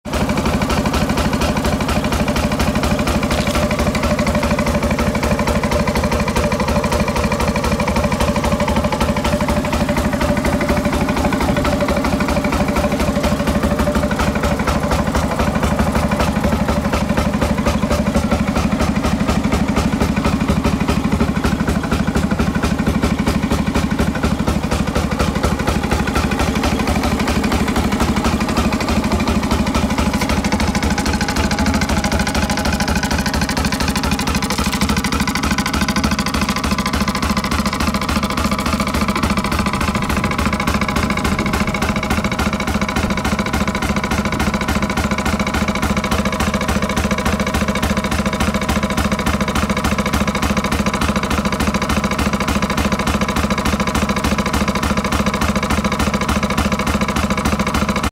Rail Speeder Walk Around
A newly-acquired speeder is inspected on its maiden voyage after some work to make it operational. The head was leaking, the big-end of the connecting rod pounded, the wiring was a nightmare and shorted out in at least three places, the alternator was missing the wiring for the regulator and the field rectifier had a bad diode. After these repairs and a good inspection, a venture onto the rails was in order.